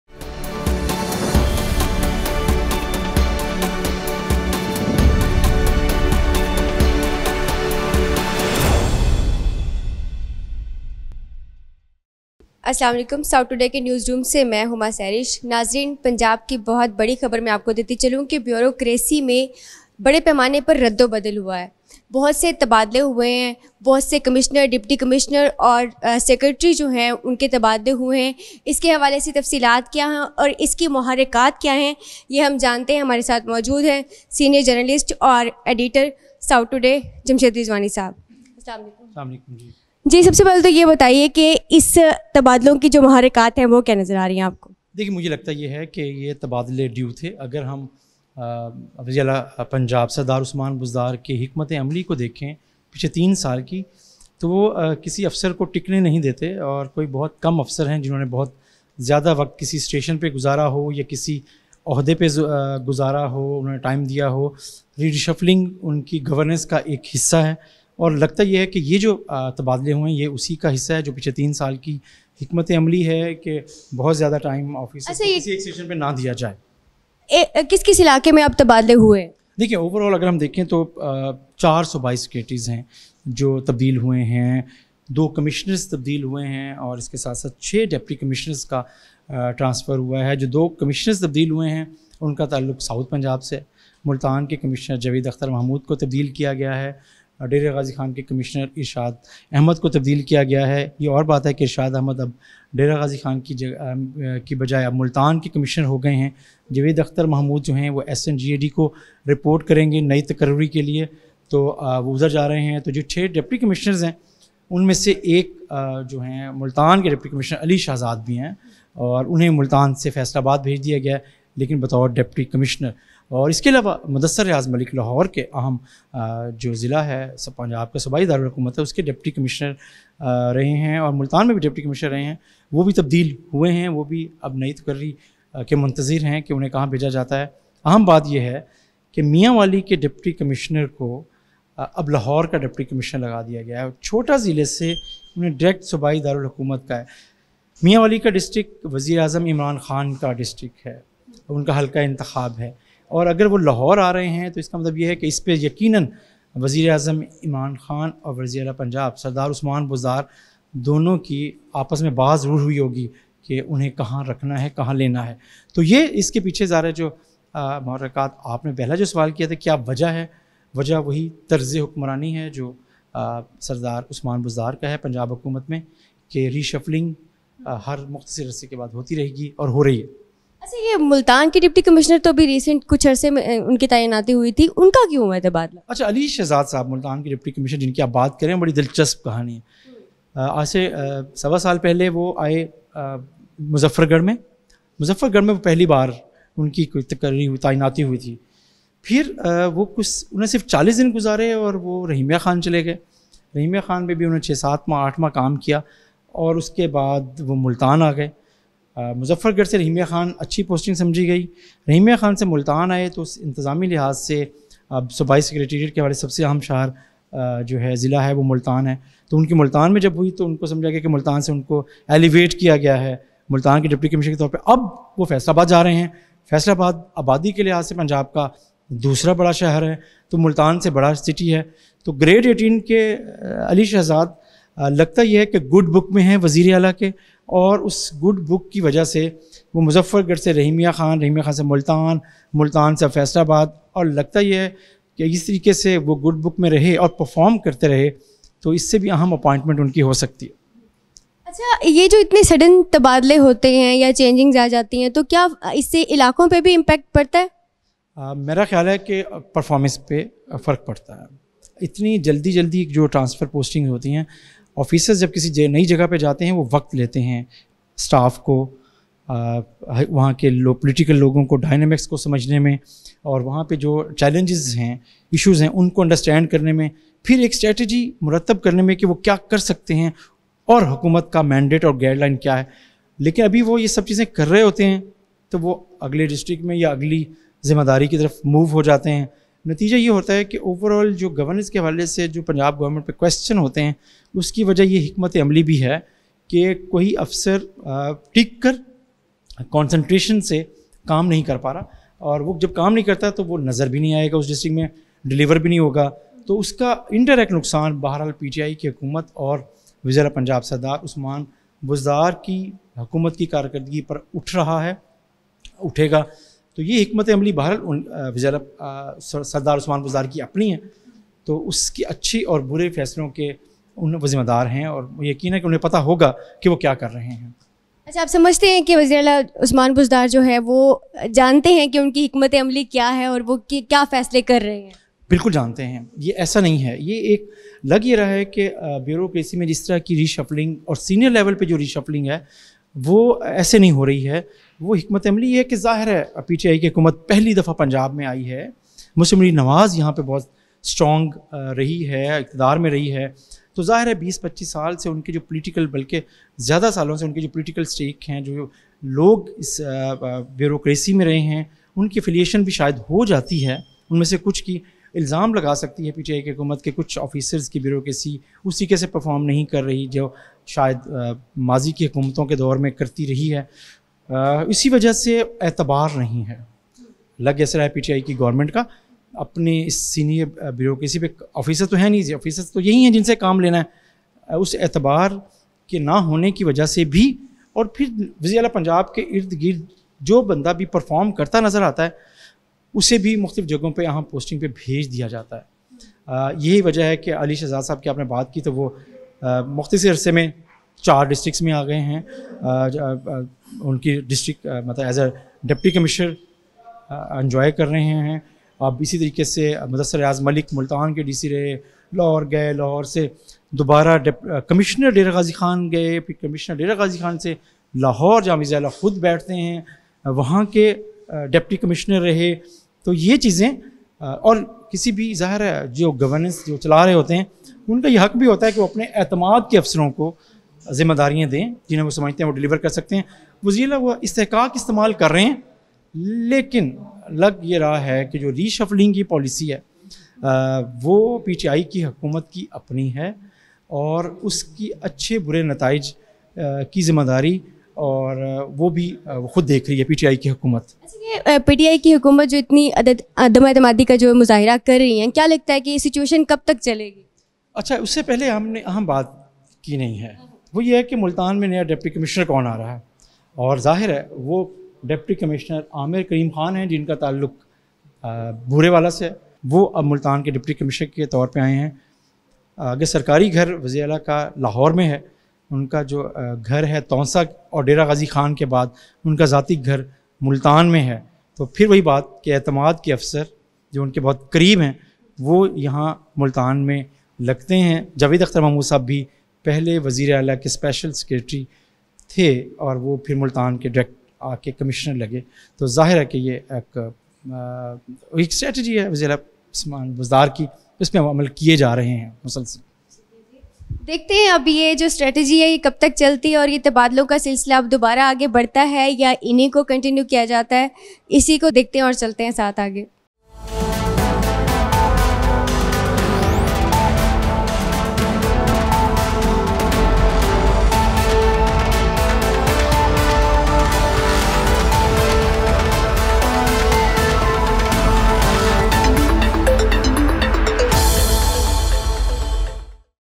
Major reshuffle in Punjab Bureaucracy
پنجاب میں اعلی سطح پر بیوروکریسی میں ردوبدل ہوا ہے،4 صوبائی سیکرٹریز،2 کمشنر،6ڈپٹی کمشنرز اور دیگر اعلی افسران کا تبادلہ اور تقرریاں کردی گئیں